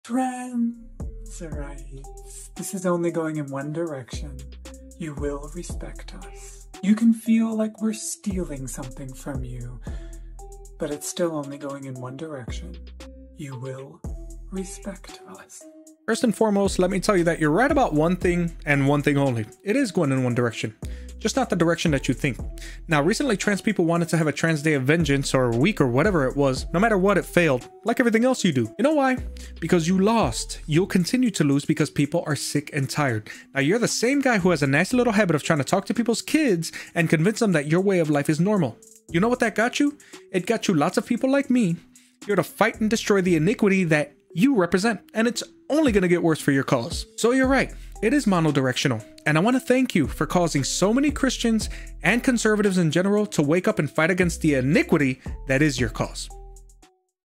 Jeffrey Marsh DEMANDS “You Will Respect Us” | Matthew 18:6
Matthew 18:6; Jesus Stated |
“If anyone causes one of these little ones—those who believe in me—to stumble, it would be better for them to have a large millstone hung around their neck and to be drowned in the depths of the sea"

Explanation and Commentary of Matthew 18:6
At first, it seems like Jesus is speaking of the little children like the one he pointed out as an example in the previous verses (Mt 18:1-5). But it is likely that he intends those who are like little children in the way that they trust in God and live the simple life of peace and assurance.

What would make “one of these little ones” stumble? Perhaps some sort of manipulation is in mind, but he could also mean a poor example or false preaching. Particularly when someone has a childlike faith that trusts God greatly, the same may be inclined to give great trust to teachers and shepherds. There is good reason why the shepherds and “blind guides” are judged most harshly in both the Old Testament and the New (Zech 11; Ja 3:1)

If church leadership leads with any sort of false motive whatever, it will be the sheep with the childlike faith who may be the most hurt. Therefore, anyone with any influence should take enormous care to prevent themselves from leading anyone astray by their teaching or example. For the sheep, it would be prudent to heed Jesus’ warning to his disciples to be “be as shrewd as snakes and as innocent as doves” (Mt 10:16).

Please click the link below to become a member in God's name, it would help a bunch! Thank you and God bless!: